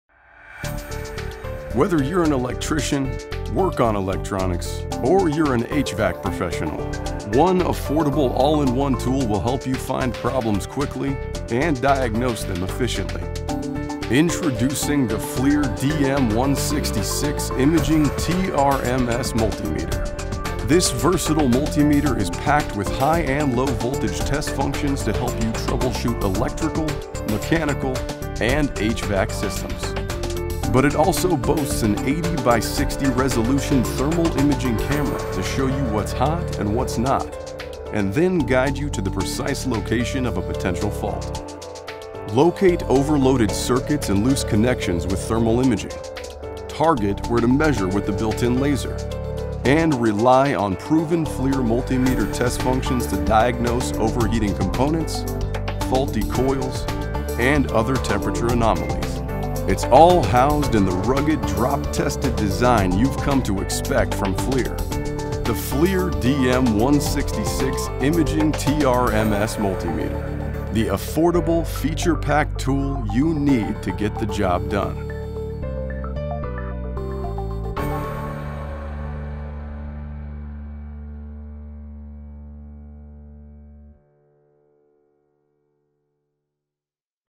FLIR DM166 이미징 TRMS 멀티미터
이 활용성 높은 IGM™(열화상측정)기능을 가진 멀티미터를 활용하여, 전기, 기계, HVAC 시스템 관련 문제를 해결할 수 있는 법을 알아보시기 바랍니다.